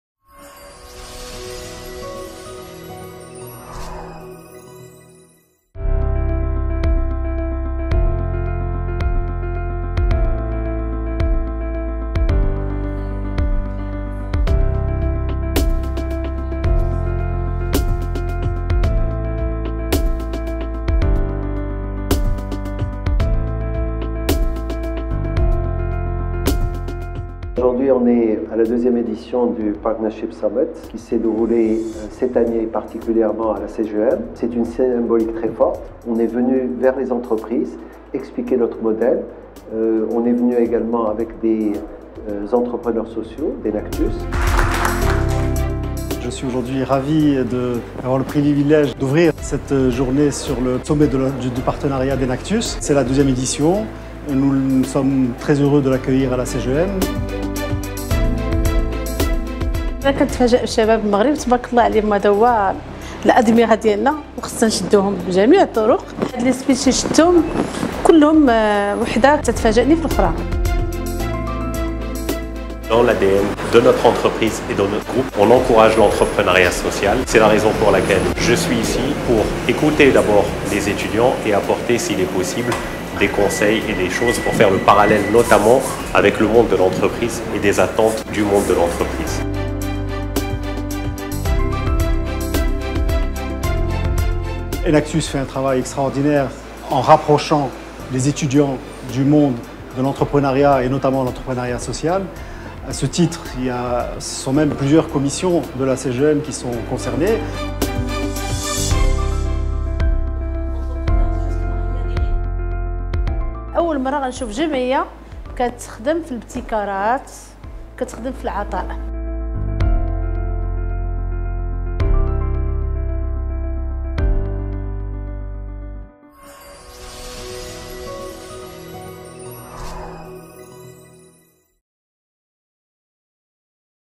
Enactus Morocco Partnership Summit - 2nd edition
Un retour en vidéo sur la deuxième édition du Partnership Summit Enactus Morocco.